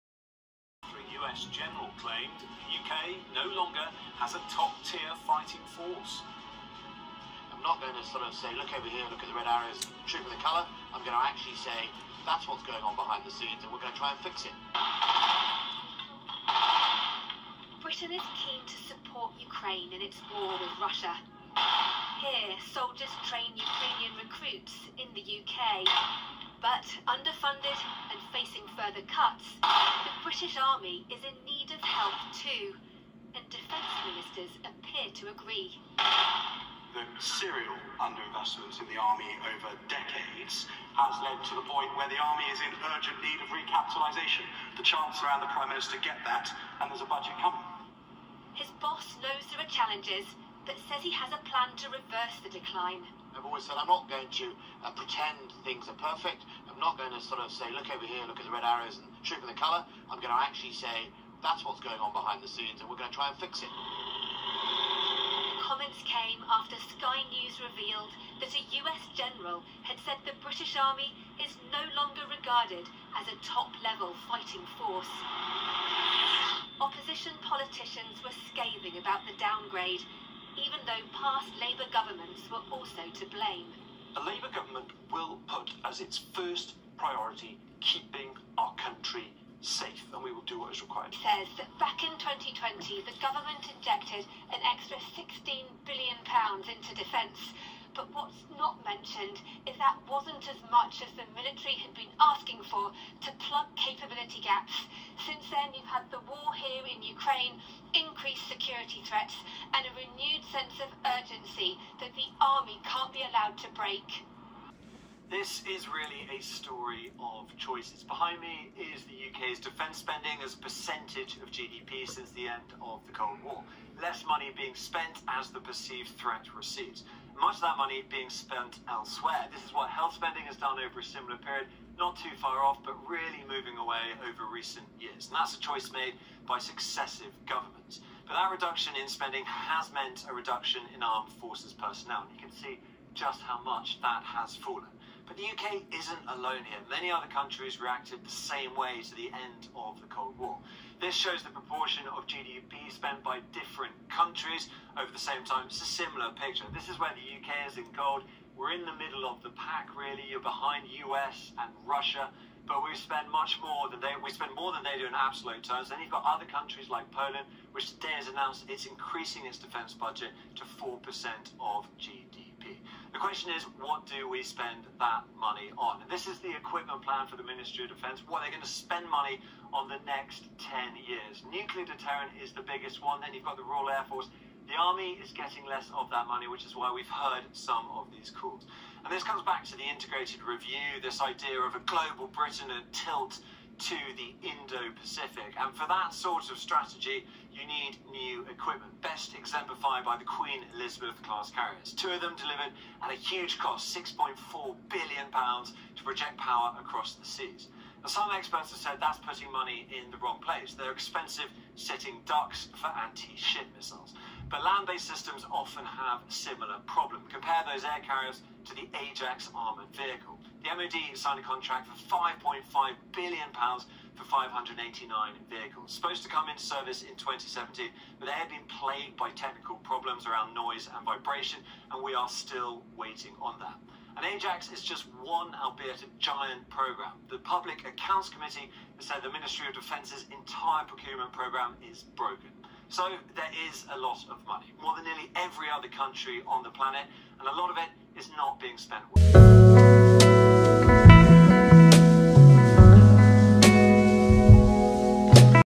British Army no longer world’s top tier fighting forces. Says, US Gen:
@British Army no longer world’s top tier fighting forces. Says, US Gen:  A senior U.S. general has recently warned U.K. Defense Secretary Ben Wallace that the British Army is no longer considered to be among the world’s top-tier fighting forces, according to a Sunday report.
The source told Wallace that decades of cuts to Britain’s military defense has eroded the country’s fighting capabilities, Sky News reports.

"Bottom line… it’s an entire service unable to protect the U.K. and our allies for a decade."  Another reportedly said there was a greater sense of urgency for the U.K. to revive its military in the wake of Russia’s ongoing military aggression against Ukraine.